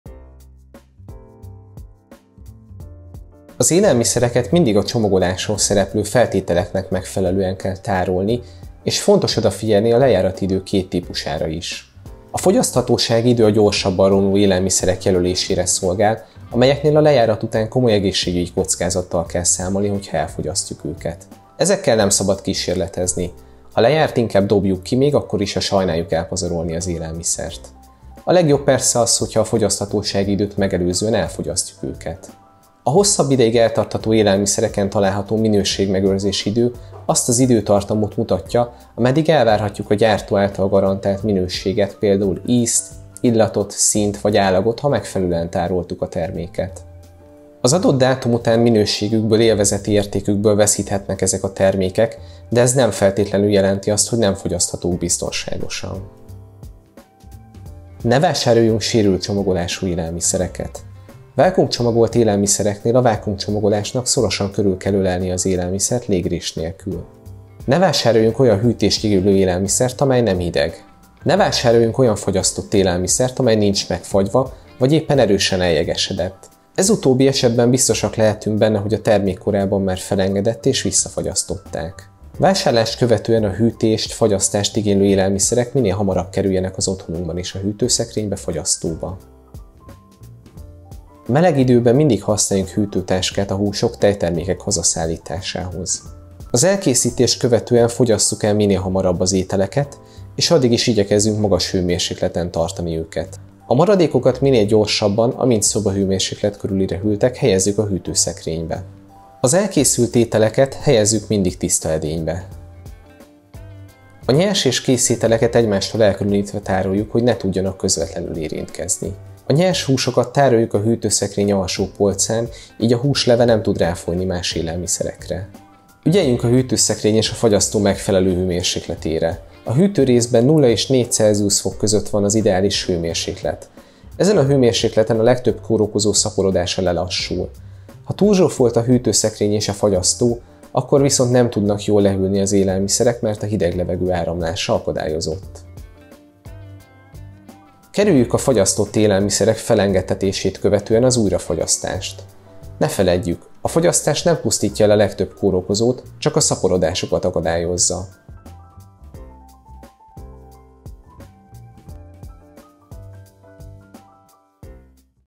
tárolás
Becslések szerint Magyarországon évente több, mint 3 millió ember betegszik meg élelmiszer eredetű megbetegedésben. Fontos, hogy bevásárlás, majd később az otthoni élelmiszer felhasználás során is gondosan járjunk el.

A Nébih az Élelmiszerbiztonsági Világnap alkalmából elindította figyelemfelhívó kampányát, melynek célja, hogy a fogyasztók tudatosságának növelése keretében felhívja a figyelmet az élelmiszerbiztonságra és a személyes felelősségükre, hiszen az élelmiszer eredetű megbetegedések legnagyobb százaléka megelőzhető.